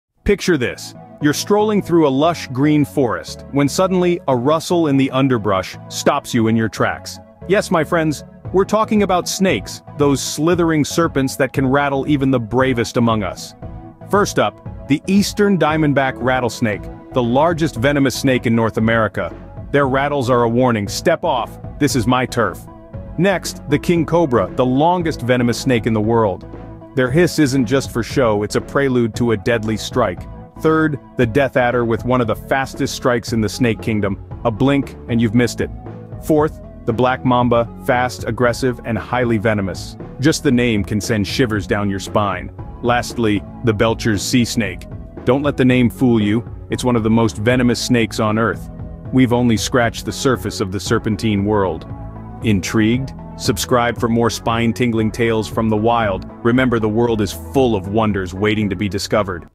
5 Snakes That Will Rattle You!
In this exhilarating video, join us on a thrilling journey as we introduce you to 5 snakes that are sure to send shivers down your spine! From the deadly Black Mamba to the powerful King Cobra, get ready to be amazed and maybe even a little rattled.